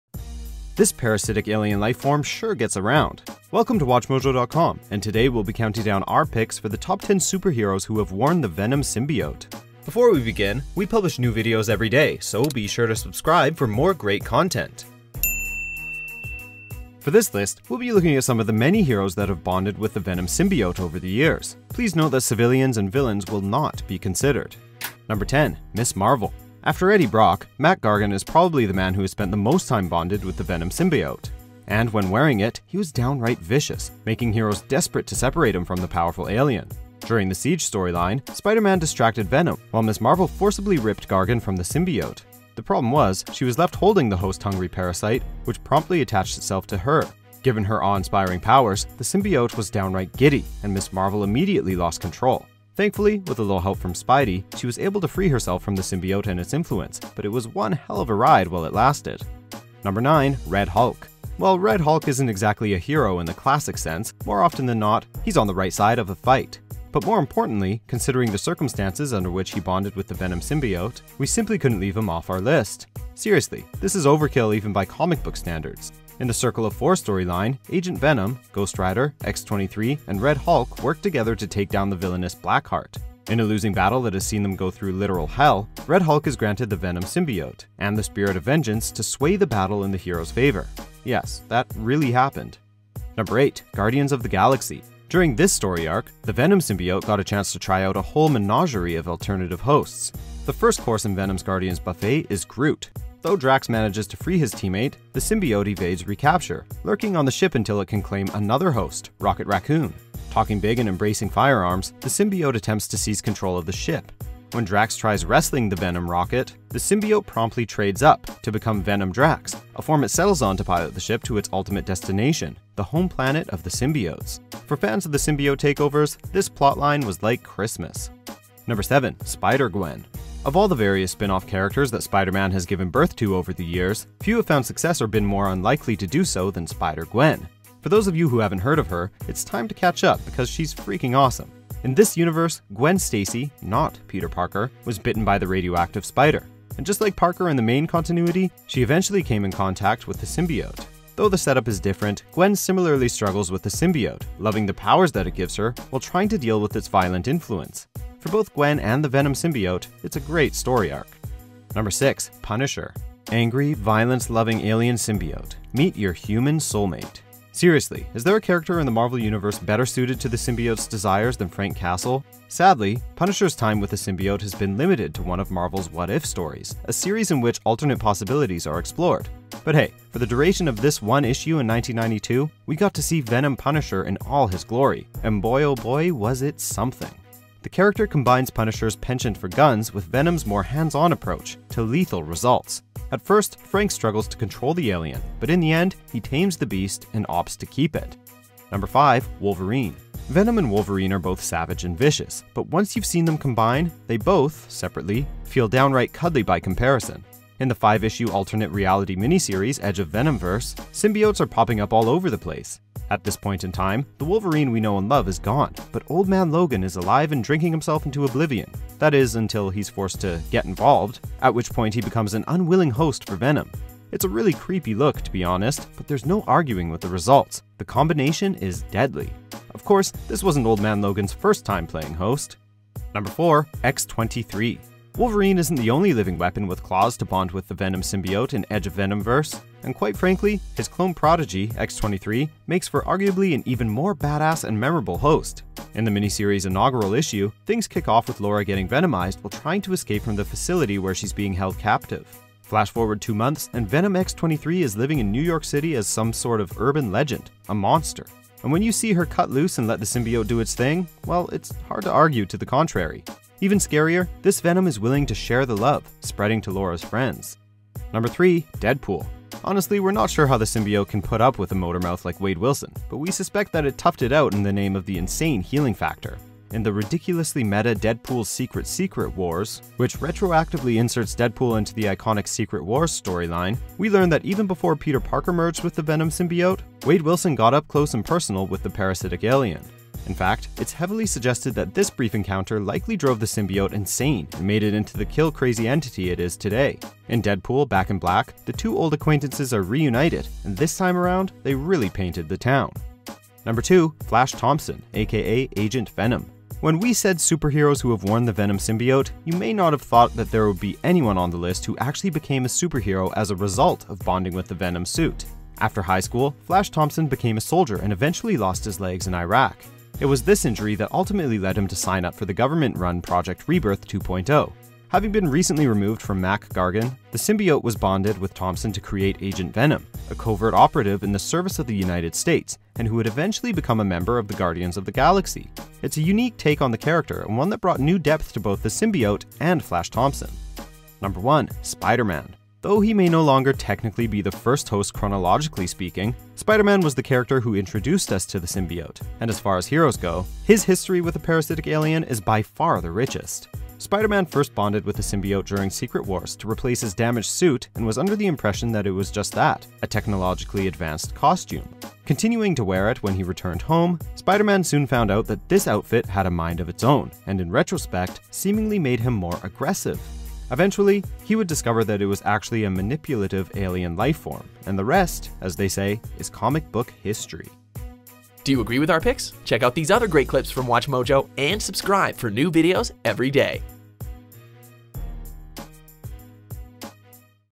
Top 10 Superheroes Who Have Worn the Venom Symbiote
What do Spider-Man, Captain Marvel, Deadpool, and Flash Thompson have in common? They've all worn the Venom symbiote, that's what! Yes, the Venom symbiote has been around the Marvel Universe, but of all the characters its bonded with over the years, this list details the most memorable times it bonded with a hero.

#10: Ms. Marvel
#9: Red Hulk
#8: Guardians of the Galaxy
#7: Spider-Gwen
#6: Punisher
#5: Wolverine
#4: X-23